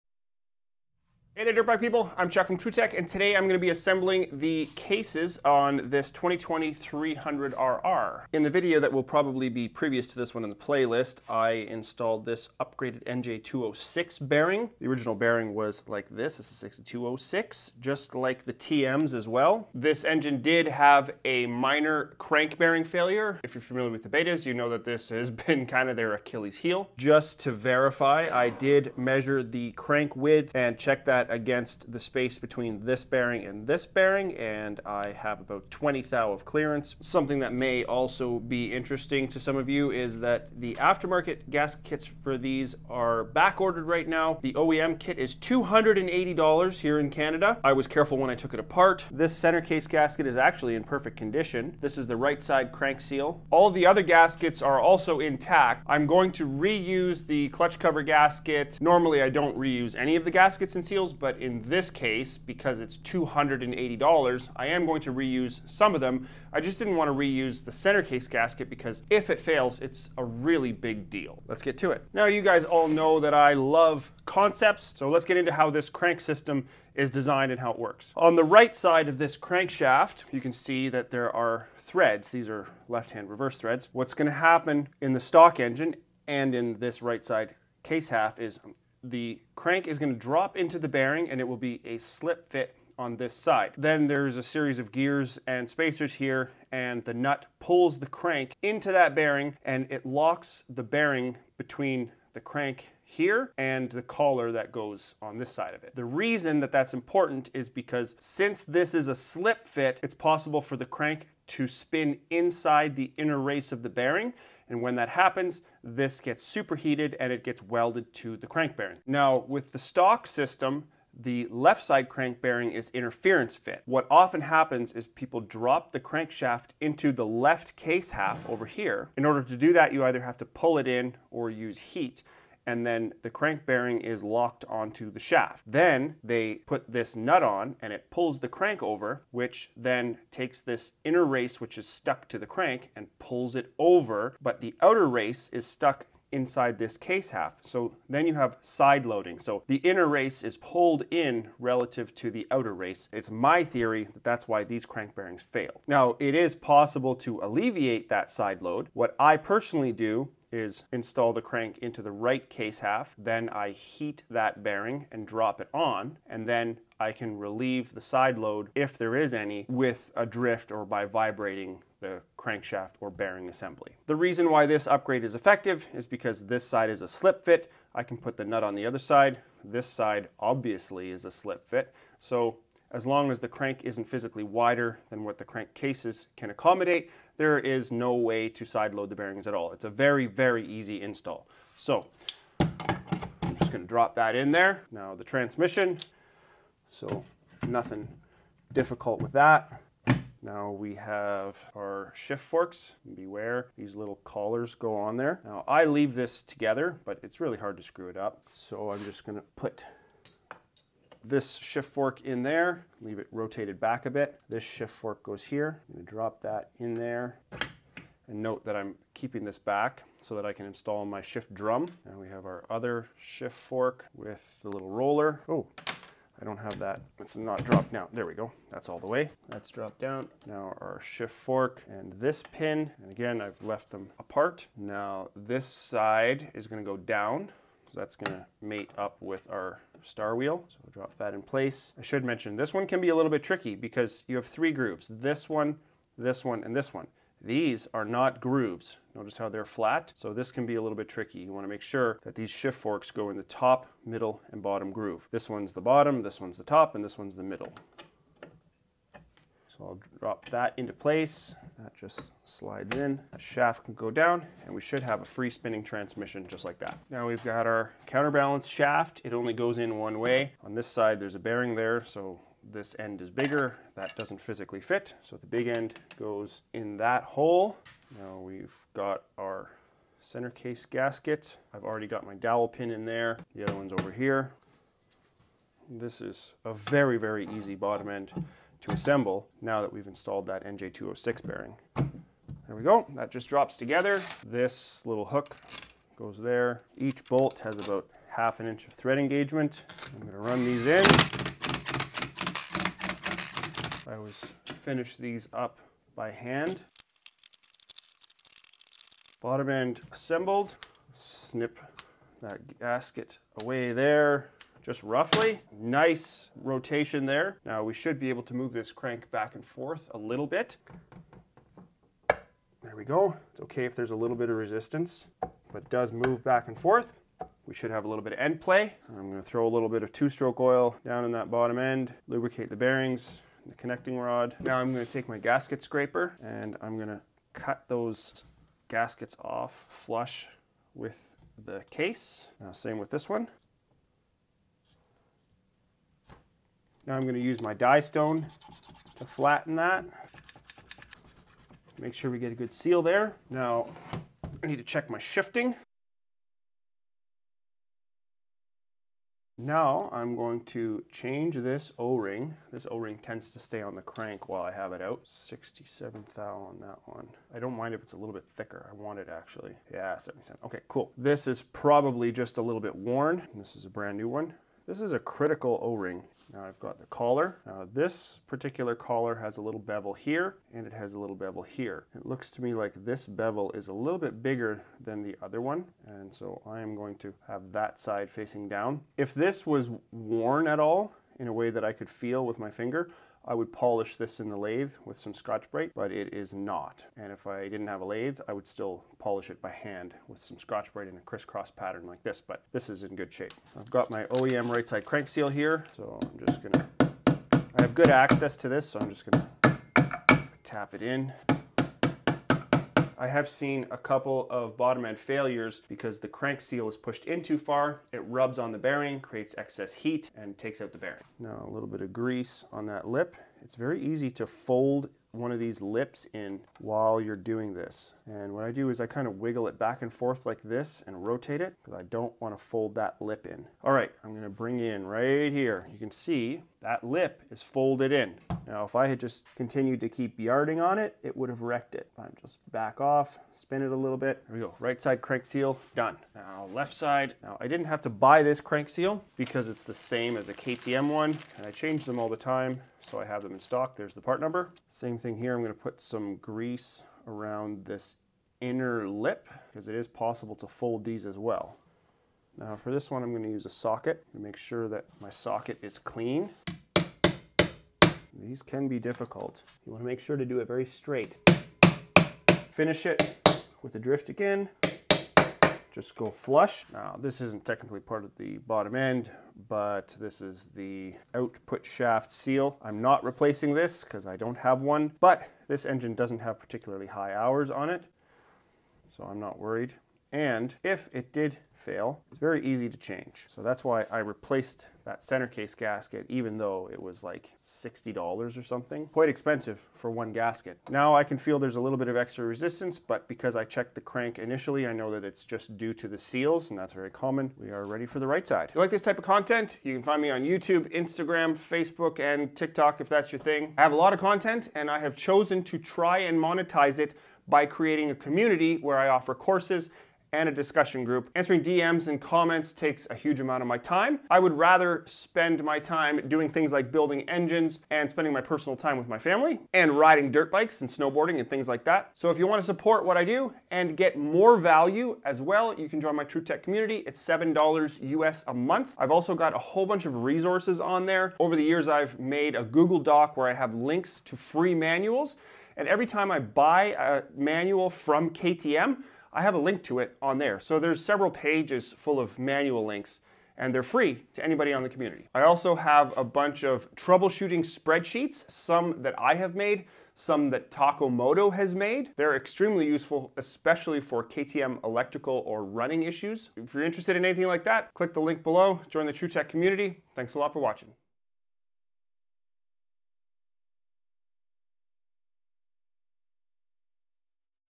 300RR Case Assembly
Here is the how & why on the Beta 300RR bottom end.